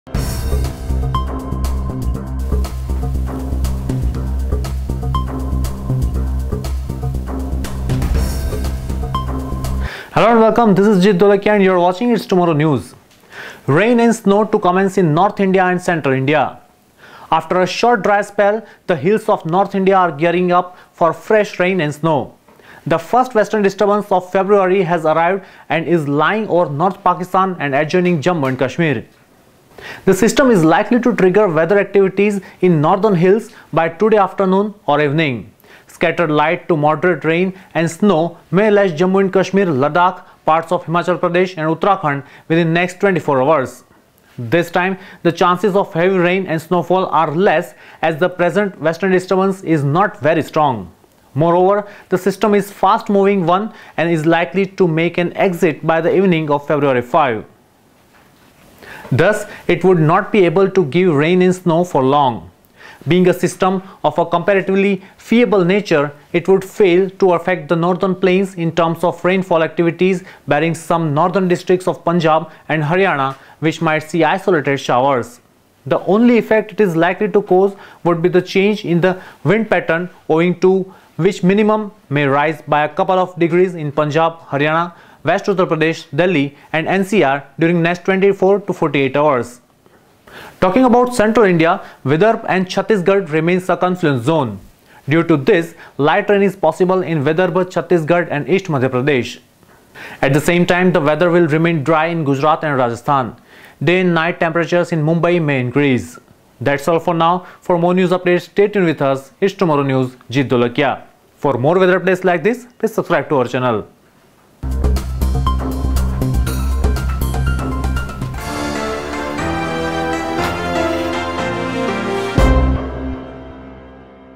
Rain and snow to commence in North India and Central India
After a short dry spell, the hills of North India are gearing up for fresh rain and snow. The first Western Disturbance of February has arrived and is lying over North Pakistan and adjoining Jammu and Kashmir. This system is likely to trigger weather activities in the northern hills by today afternoon or evening.

Source